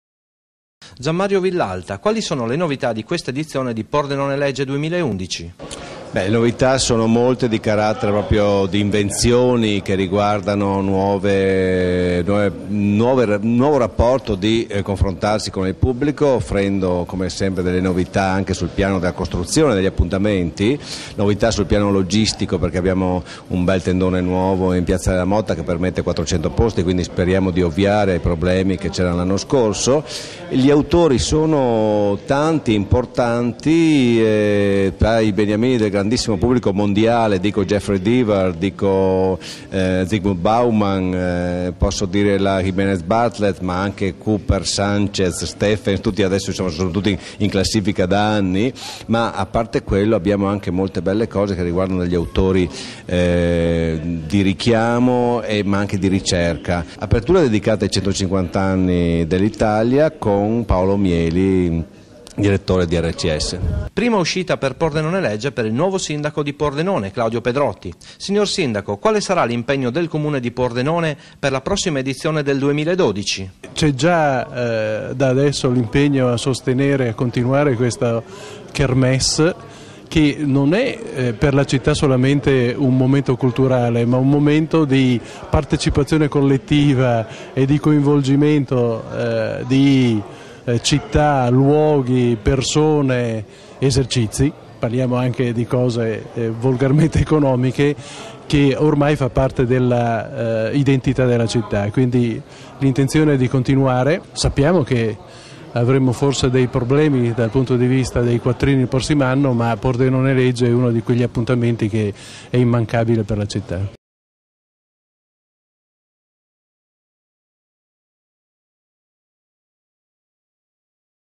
Presentazione Pordenonelegge 2011
DAL 14 AL 18 SETTEMBRE 2011 PORDENONELEGGE.IT FESTA DEL LIBRO CON GLI AUTORI.

Si rinnova l'appuntamento con pordenonelegge, la Festa del Libro con gli Autori: è l'undicesima edizione di una tra le più attese manifestazioni dell'agenda culturale italiana.